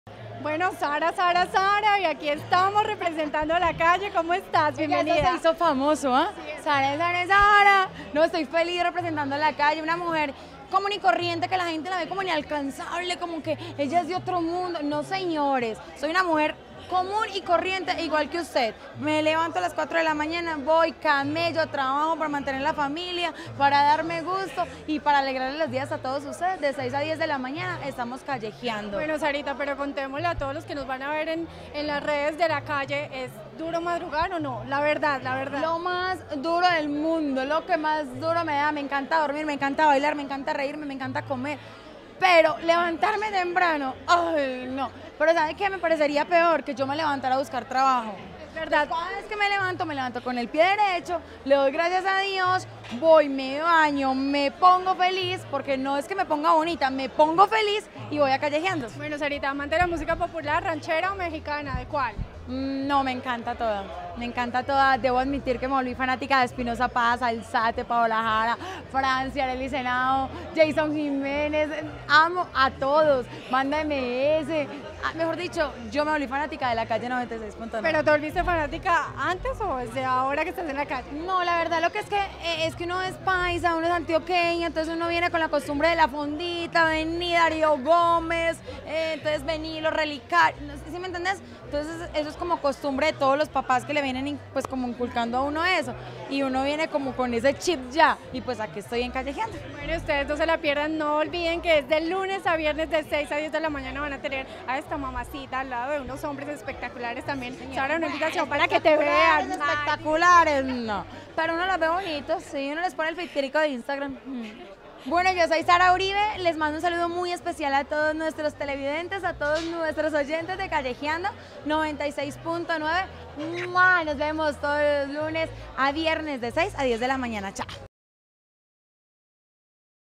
La presentadora Sara Uribe contó cómo ha sido su experiencia en ‘Kallejiando’
Durante el lanzamiento oficial de La Kalle, la presentadora admitió que se volvió fanática de la música popular y mexicana.

No olvides verla de lunes a viernes en el programa que alegra tus mañanas ‘Kallejiando’.